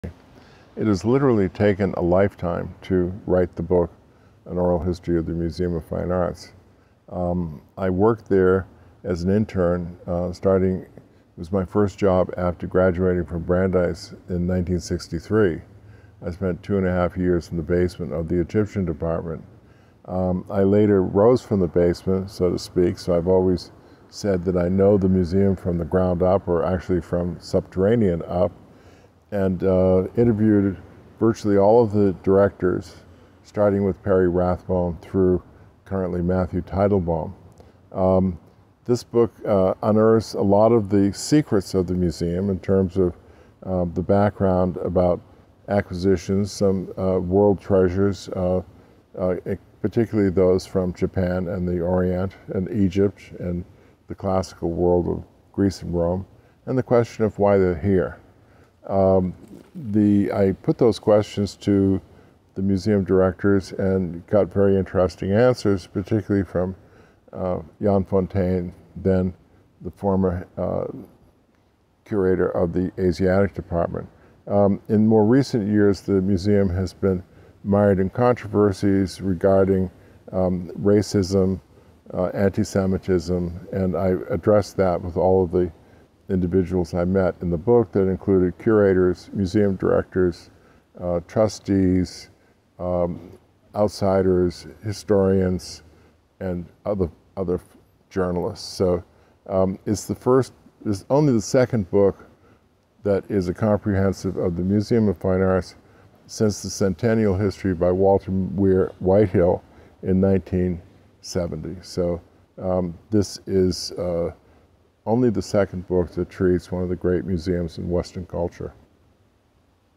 Charles Giuliano - Museum of Fine Arts Boston
Author Charles Giuliano discusses his seventh book, Museum of Fine Arts Boston: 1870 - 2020 An Oral History